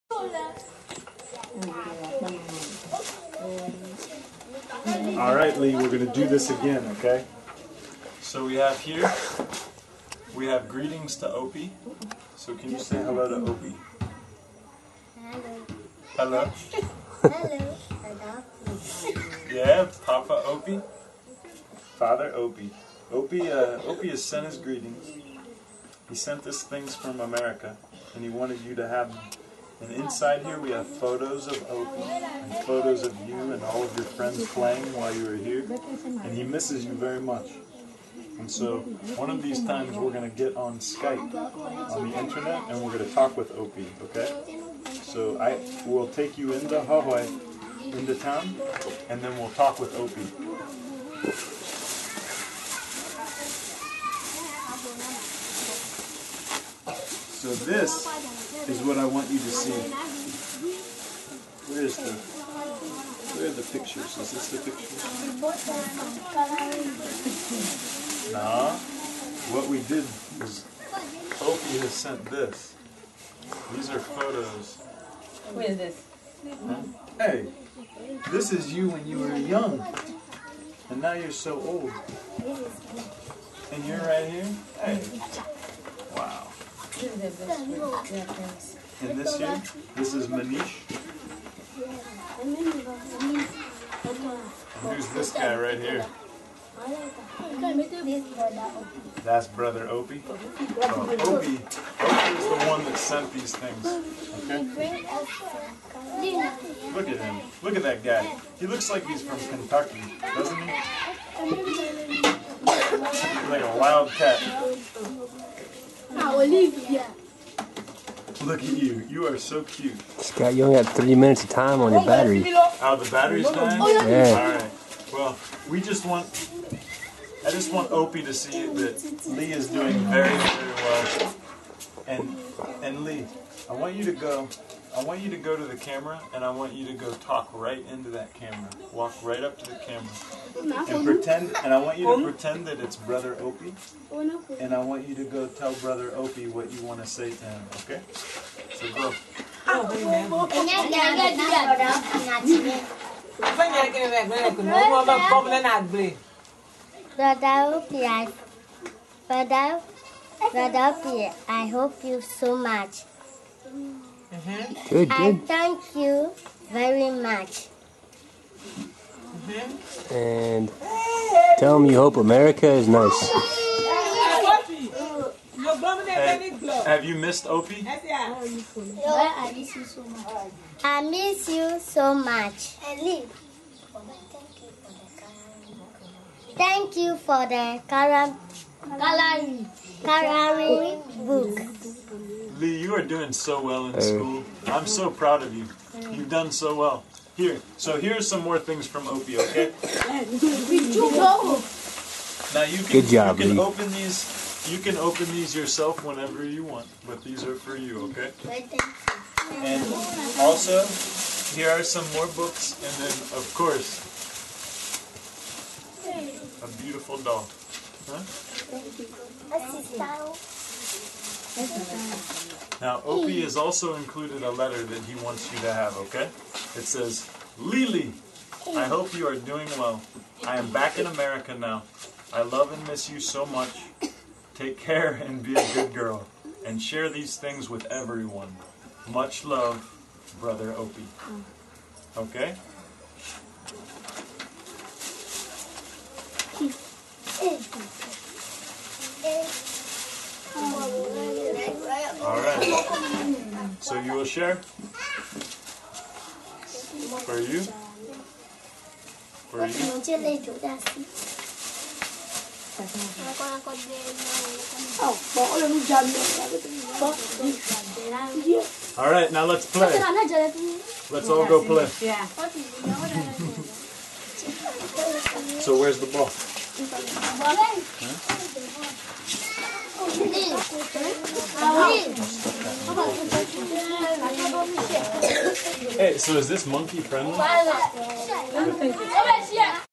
A message to Opie Mcguire from Ghana
Opie McGuire is a hero to many.  He served alongside Peace Corps Volunteers in Uganda and most recently in Ghana.  As a volunteer he saved a young girls life.  The details are long, painful and best told by Opie himself, but this video depicts the co-founders of Our Talking Hands, Scott and Promise presenting this little girl with gifts sent by Opie.

The video is meant for Opie, but stands as a testament for all to see that one person can have a profound impact on the lives of those around them.  Thanks be to Opie for all you have done to inspire the rest of us.  Your legend lives on, and so must the story of a man that fought against popular opinion and did what was correct.